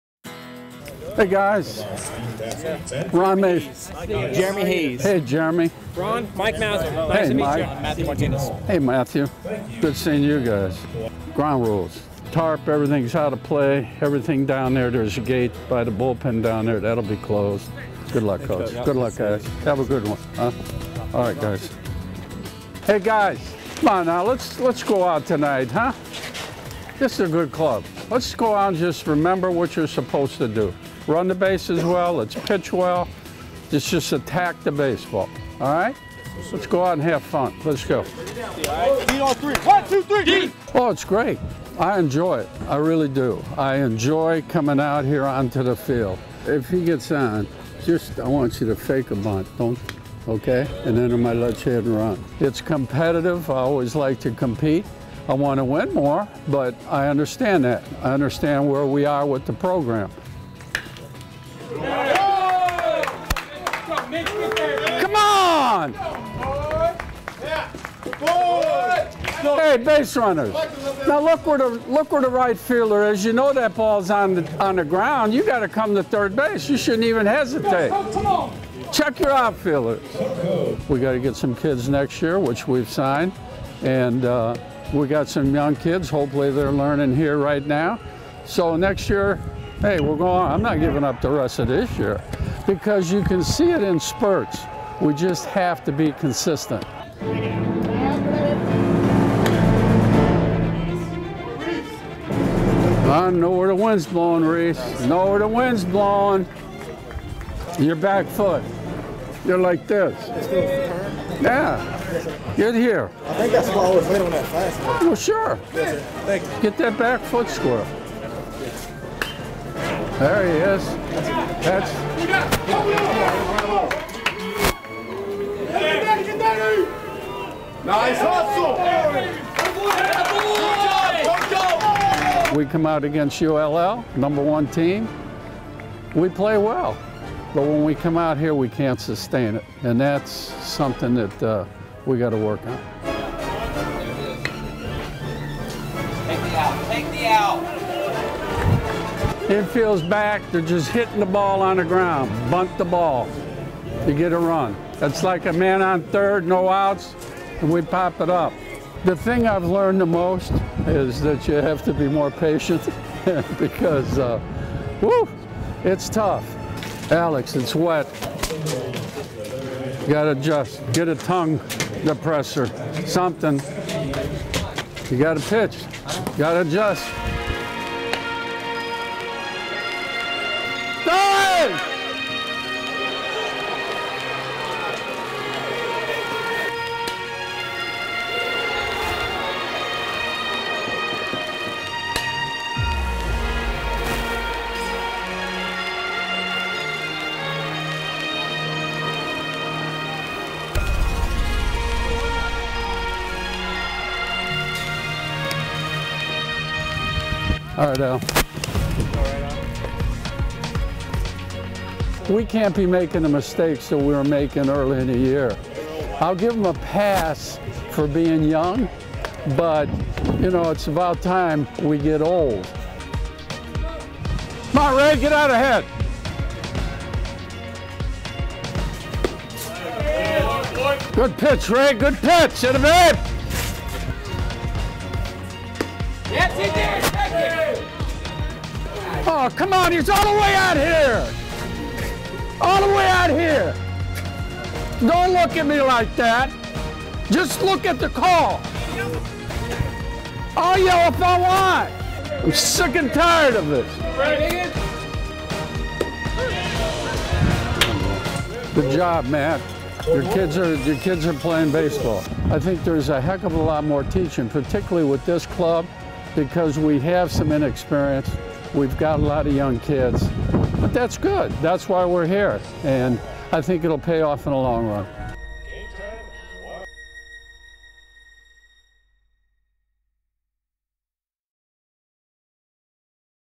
Ron Maestri All-access
For the first time in his career, UNO's Ron Maestri was mic'd up for a game.